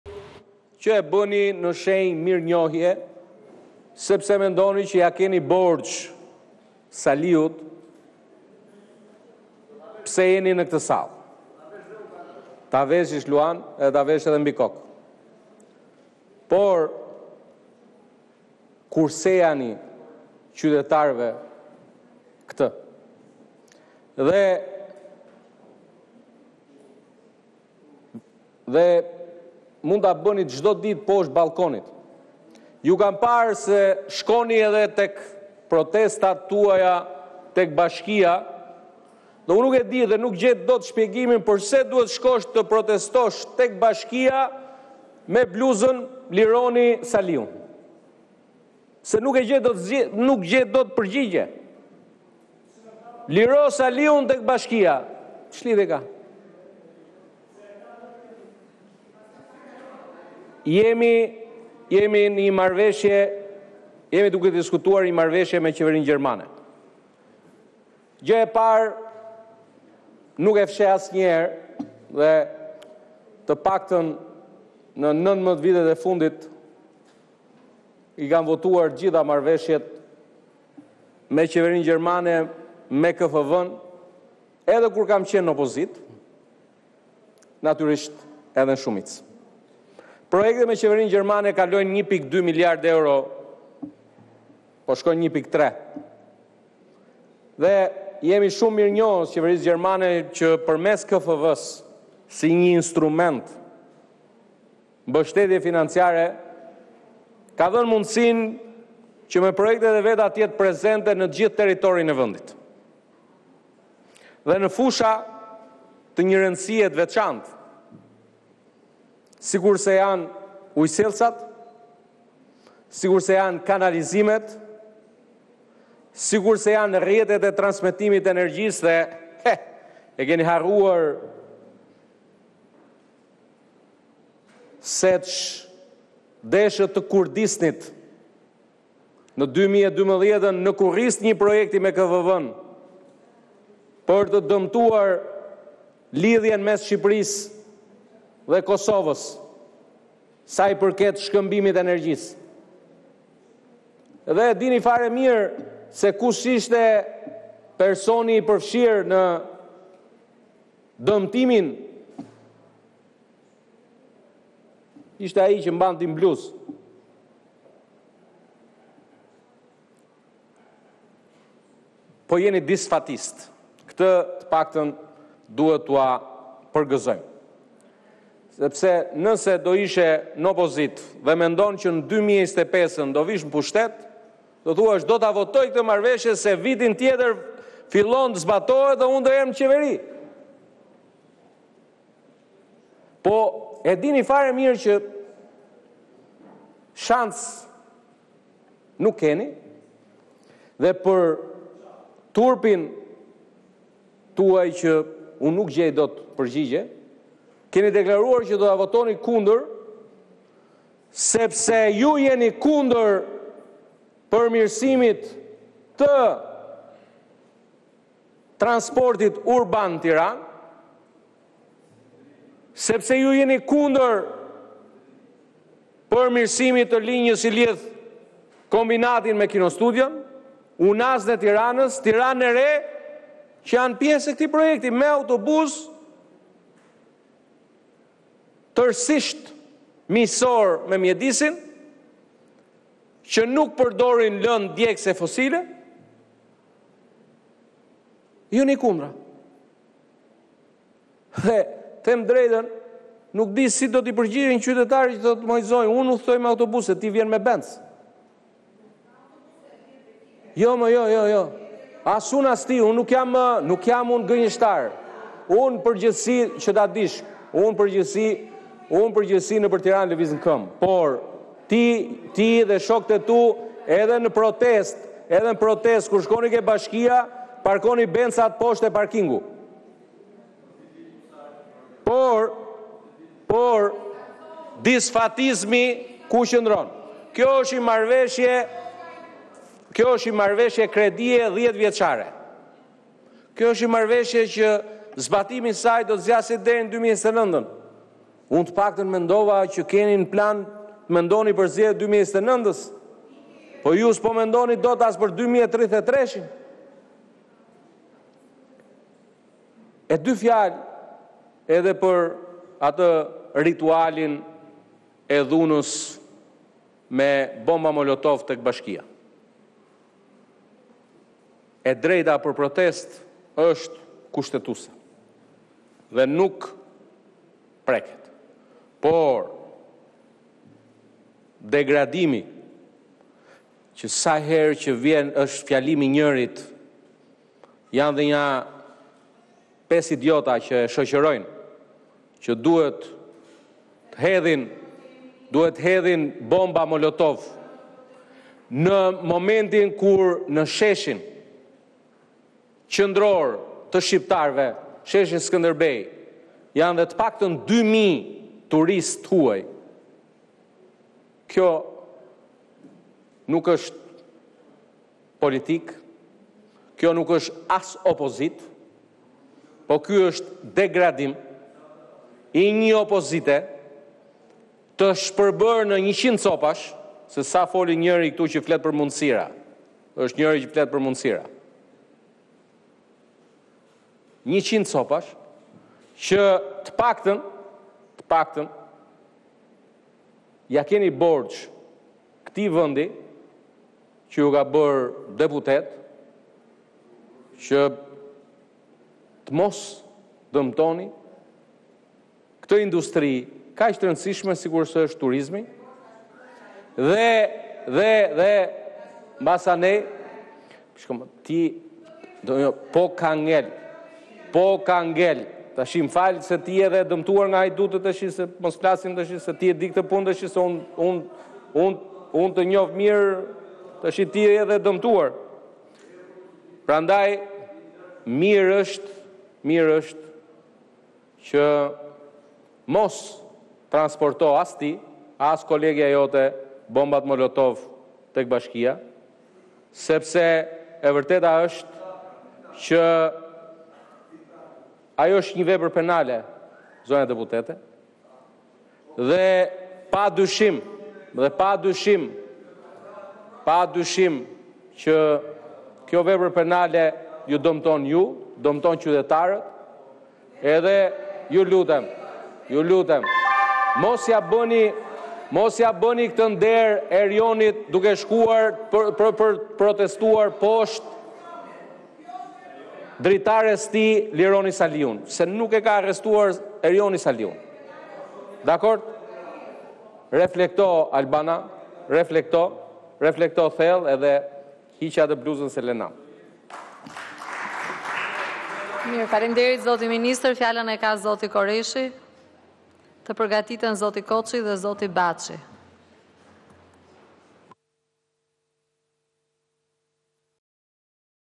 "Parkoni Benzat, 5 idiota hedhin bomba molotov", Balla: Saliun nuk e ka arrestuar Veliaj
Opozita ka deklaruar se do të votojë kundër marrëveshjes së qeverisë me qeverinë gjermane për transportin publik urban në kryeqytet. Ministri i Brendshëm Taulant Balla iu dha një përgjigjie, duke ju kujtuar zgjedhjet e 2025-ës.

“Jeni disfatistë! Këtë të paktën ua përgëzojmë, se nëse do ishe në opozitë dhe vitin tjetër janë zgjedhjet, do thuash do ta votoj këtë marrëveshje sepse vitin tjetër fillon zbatimi dhe unë jam në qeveri. Por e dini se keni 0 shans! Kjo është një marrëveshje kredie 10 vjeçare, unë të paktën mendova se keni në plan të mendoni për zgjedhjet e 2029-ës, por ju s’po mendoni as për 2033-in.”

Më tej ai ironizoi protestën para bashkisë së Tiranës.

“Mendoni se ia keni borxh Saliut pse jeni në këtë sallë. Ta veshësh luan, edhe ta vësh edhe mbi kokë. Nuk e kuptoj pse duhet të shkosh të protestosh te Bashkia, me bluzën “Lironi Saliun!” “Liro Saliun te bashkia, ç’lidhje ka? Unë në përgjithësi nëpër Tiranë lëviz në këmbë, por ti dhe shokët e tu kur shkoni edhe në protestë te bashkia, parkoni “Benzat” te parkingu. E drejta për protestë është kushtetuese, por degradimi, që sa herë vjen është fjalimi i njërit dhe nja 5 idiota që e shoqërojnë që duhet të hedhin bomba molotov. Kur në sheshin Skënderbej janë dhe të paktën 2 mijë turistë të huaj, kjo nuk është politikkë, as opozitë, por degradim i një opozite të shpërbërë në 100 copash. Se sa foli njëri për mundësira…Të paktën ja keni borxh këtij vendi që u ka bërë deputetë që të mos dëmtoni këtë industry kaq të rëndësishme sikurse është turizmi dhe më pas…”

“Hiqe atë bluzë, se le nam!”, kështu iu drejtua deputetes së PD Albana Vokshi.

“Mirë është që mos transportosh as ti, e as kolegia jote bombat molotov te bashkia se ajo është një vepër penale dhe padyshim që kjo vepër ju dëmton ju, qytetarët…Ju lutem! Mos ia bëni këtë nder Erjonit duke shkuar poshtë dritares së tij e duke thirrur “Lironi Saliun”, se nuk e ka arrestuar ai”, shtoi Balla.